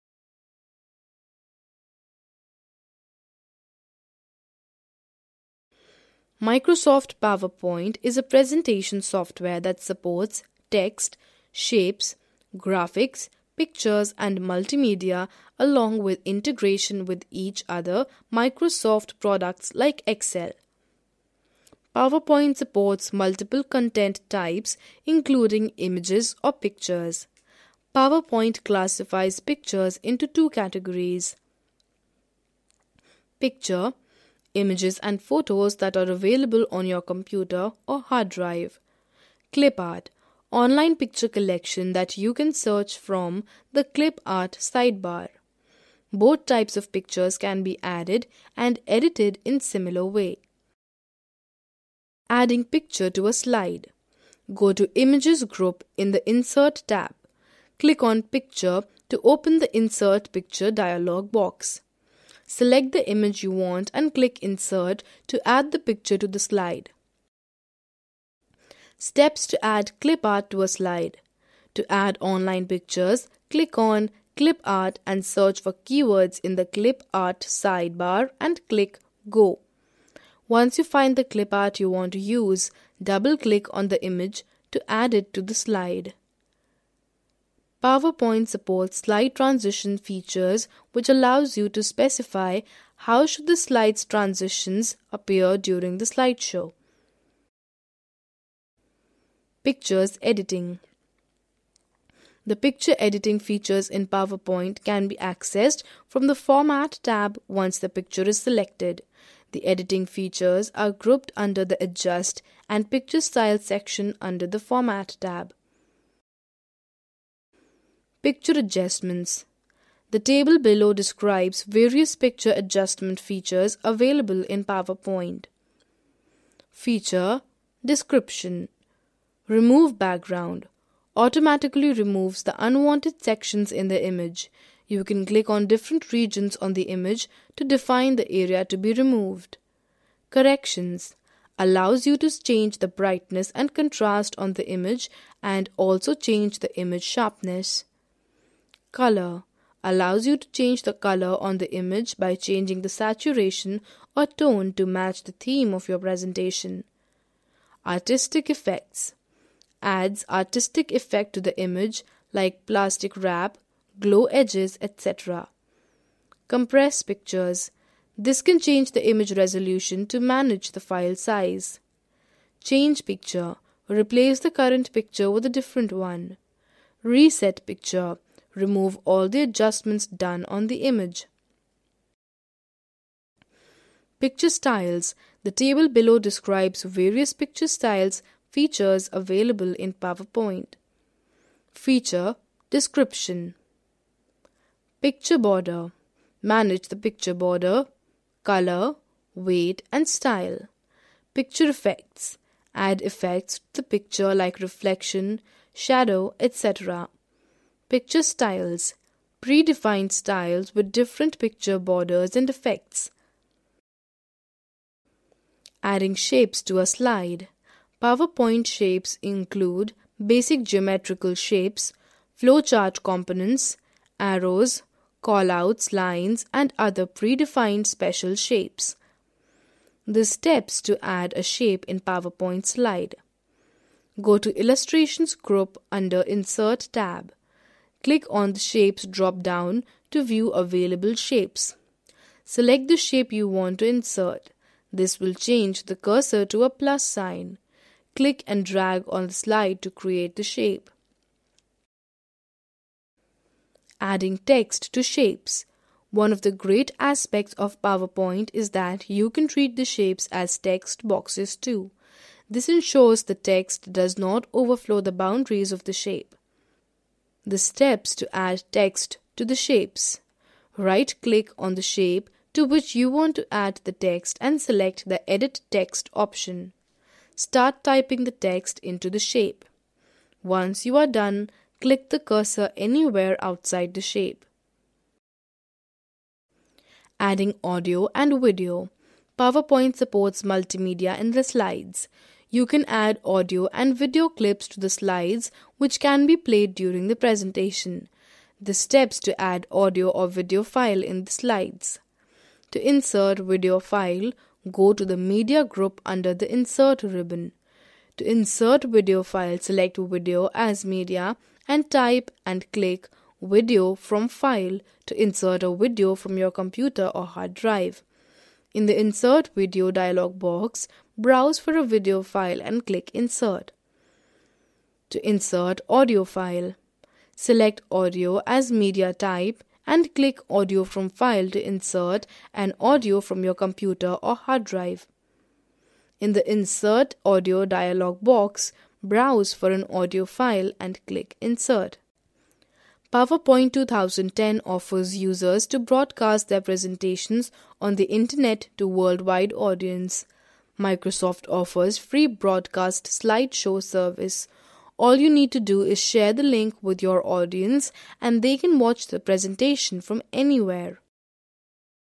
MS POWERPOINT 2010 class-6
This video is from Kriti Educational Videos. MS PowerPoint 2010 is presentation software that supports graphics, text, shapes pictures and multimedia along with integration with each other Microsoft products like MS Excel. MS PowerPoint 2010 classifies pictures into two categories, pictures and images available on our computer and we can also select images from the online collection of clipart. One can learn about adding different things in MS PowerPoint 2010.
 Contents:
1. Features of  MS Power point 2010
2. Adding picture & clipart to a slide
3. Picture adjustments and styles
4. Adding shapes and text
5. Adding Audio and video